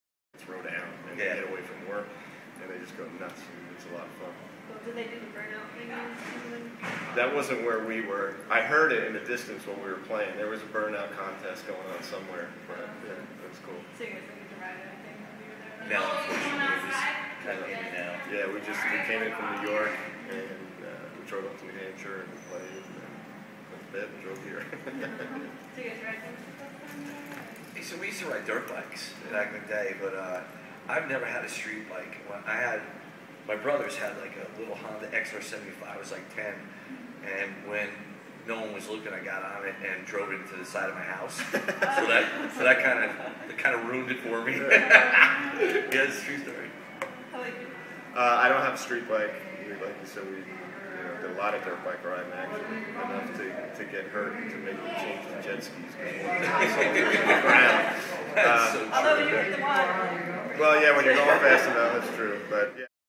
Rachel Bolan and Dave Sabo (Skid Row) at the Utica Hotel 2011
Talking Racing and Dirtbikes with Rachel Bolan and Dave Sabo of Skid Row. 2011